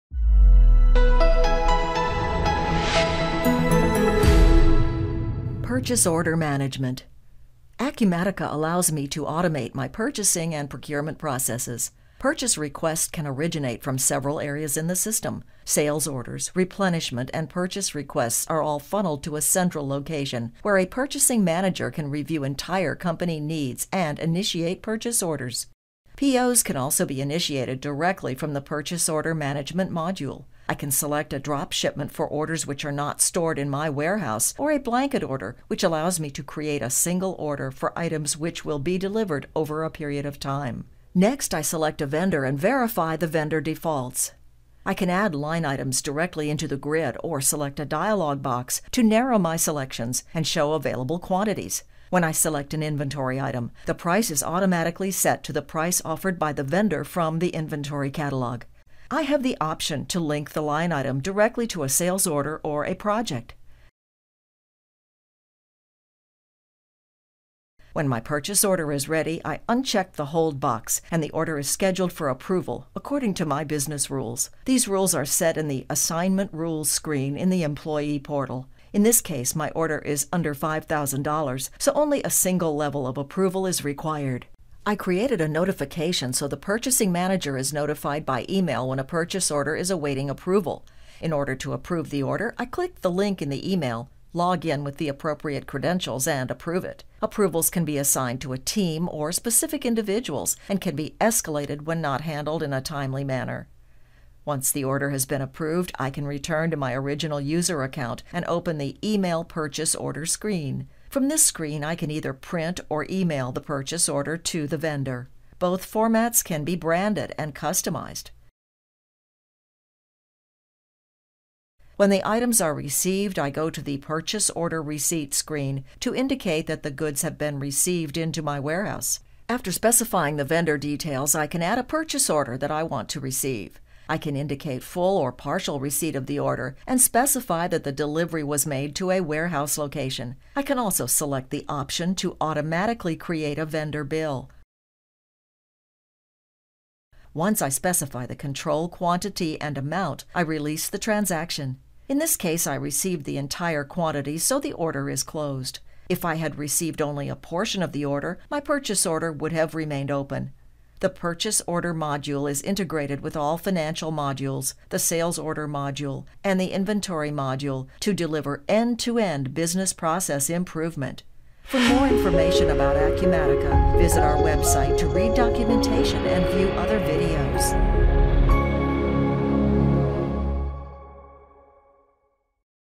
Acumatica Purchase Order Management
Overview of Purchase Orders using Acumatica Cloud ERP software. Shows end to end flow including placing order, approving PO, workflow notification, and receipt of order.